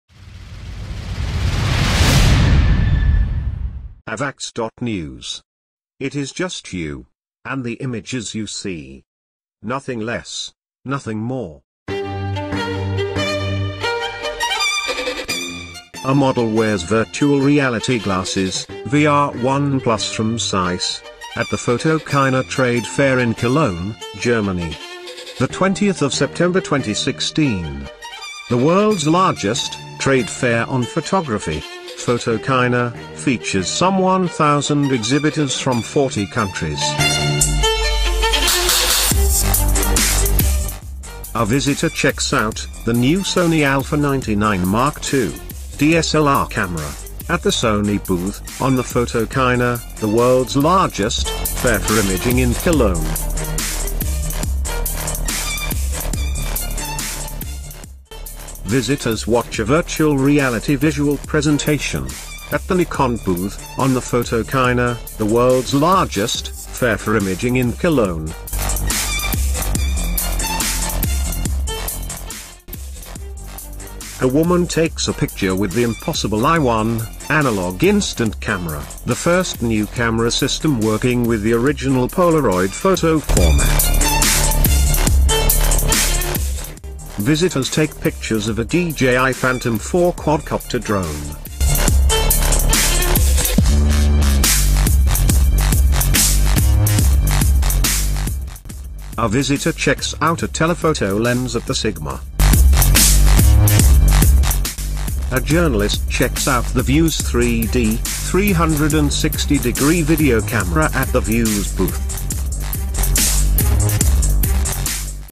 Photokina 2016 in Cologne, Germany
Photokina 2016 is the world’s leading trade fair for the photo, video and imaging sectors. Every two years it inspires as a popular meeting point and an important provider of impulses.